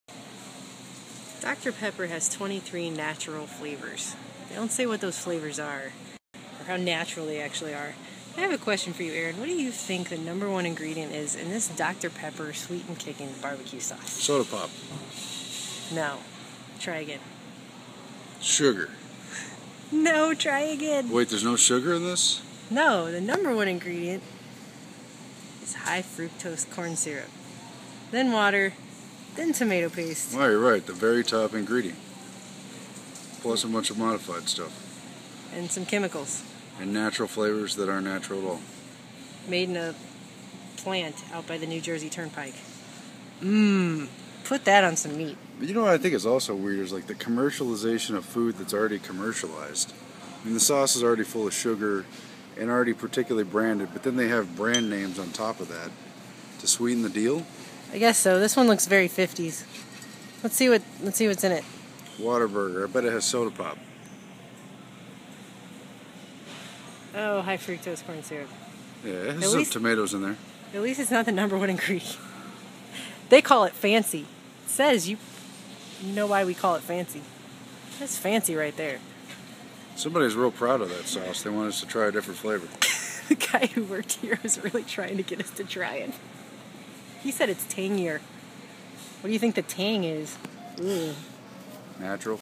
What's in That 'Secret' Sauce?
Some special sauces are made even more 'special' with your favorite corporate logos, contributing toward even more high fructose (GMO) corn syrup, plus tons of other "natural" and artificial flavors. And chemicals. Enough to make anyone chronically ill.

While sugar itself is plenty dangerous and should be consumed minimally and with restraint, a number of substitutes have their own issues that may even out do the harm of plain sugar.

Natural News covers just some of the issues with HFCS, noting its common usage in popular name brand barbeque sauces:

What is HFCS?
High fructose corn syrup is a pseudo-food borne out of the marriage of convenience, using government-subsidized corn with industrial chemical food processing methods. Mercury is used in order to transform corn starch into corn syrup. Testing has revealed that this toxic chemical may remain present in the resulting "food" product.

A toxic embalming chemical, glutaraldehyde, is also used to concoct corn syrup. The chemistry games played by food processors result in people consuming extraordinarily large amounts of a form of sugar, fructose, which the human body cannot digest as easily as sucrose. Research is steadily mounting, showing the health consequences of consuming HFCS include fatty liver, increased abdominal fat, higher triglyceride levels, increased tooth decay, anemia, osteoporosis, ADD and more.

Moreover, the makers of the documentary King Corn demonstrate the complicated technology and many steps needed to transform yellow dent #2 corn (different from corn-on-the-cob) into the high fructose corn syrup sweetener that has become so pervasive in our foods, including the use of a number of dangerous and/or toxic enzymes and ingredients.
Must be why this video goes in the "science and technology" category...Yum!

The next time you want to pour a chemical soup all over your meats, head on down to your average grocery store...